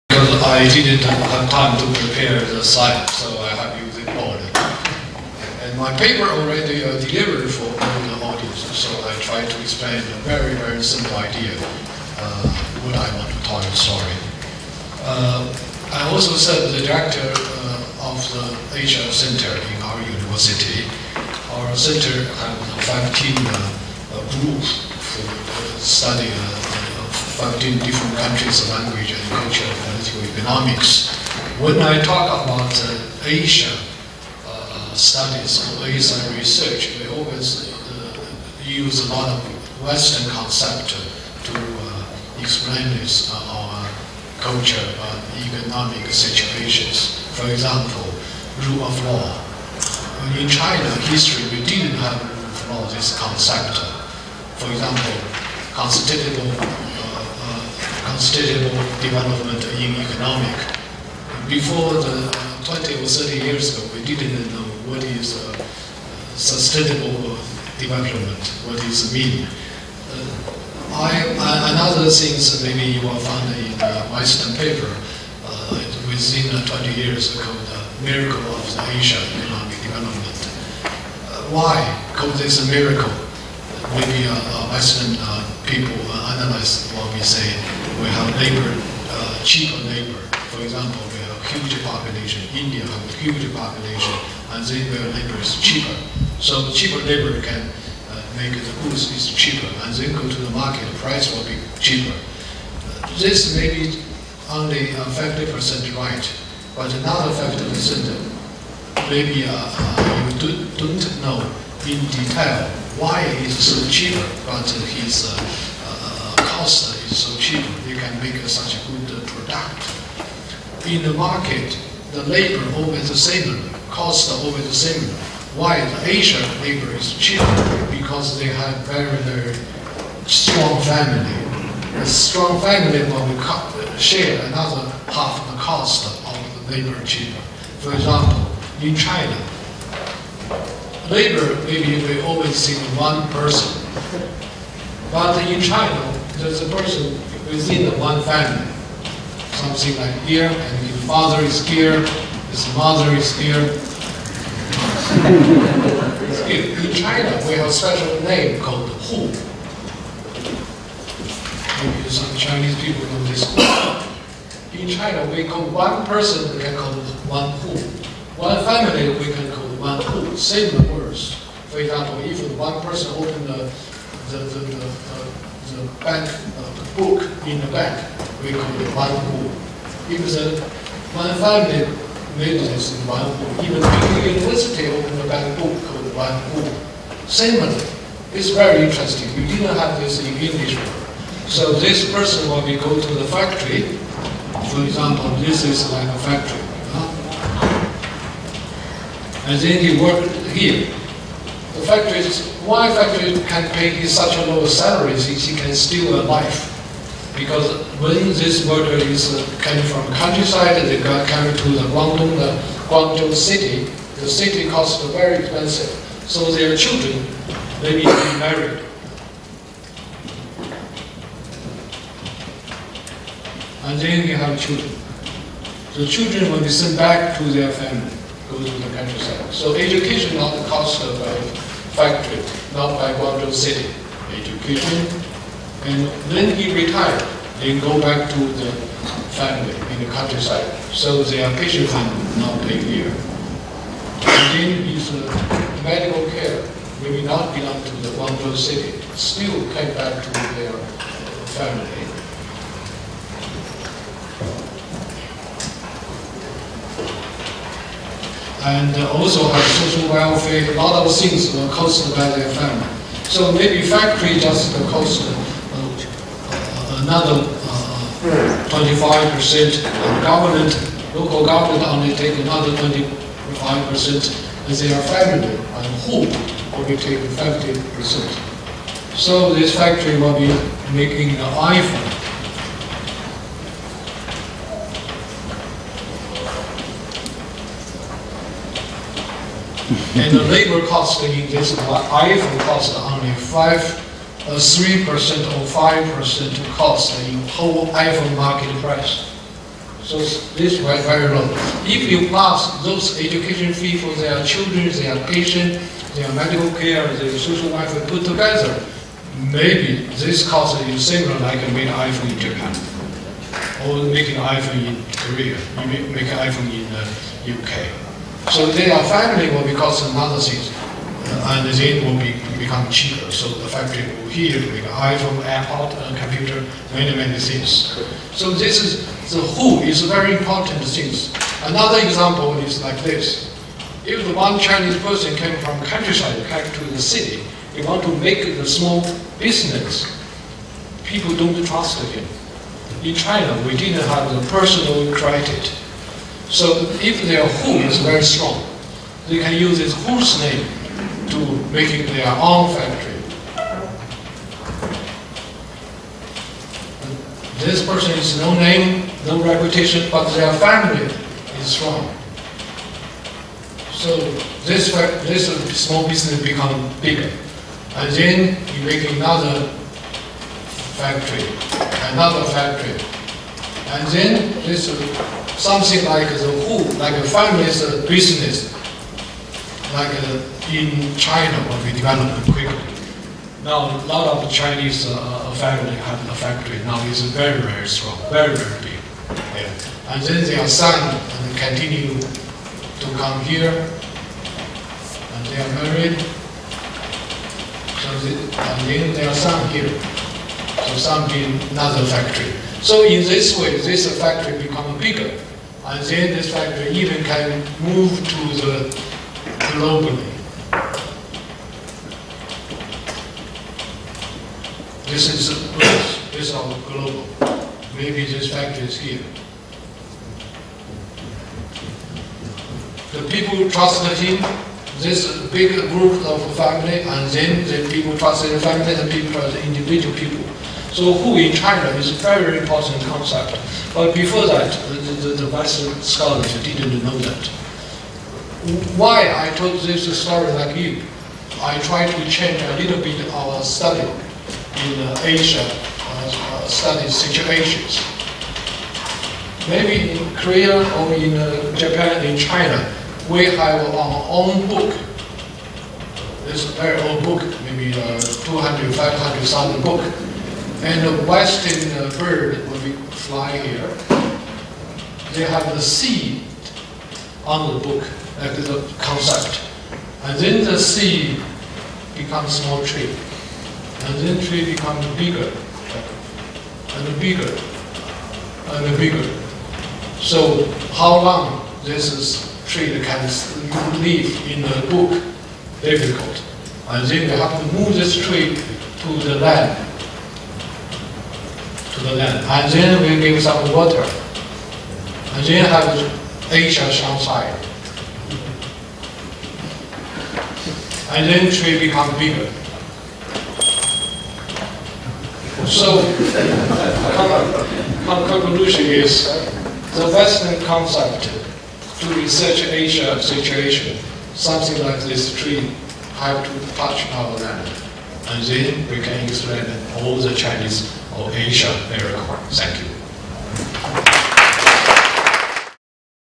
[#1-4]Zhipan Wu, "Family Concept, Family Enterprises ..."
[#1-4]Zhipan Wu, "Family Concept, Family Enterprises, East-Asia Miracle and Legislature Coperation in East Asia"

Prof Zhipan Wu, Executive Vice President, Peking University

Session 1: Emerging Questions in Asian Studies

"New Horizons of Asian Studies : Asia Research Directors Conference"
Seoul National University Asia Center

Seoul National University Asia Center Samik Hall
Seoul, Korea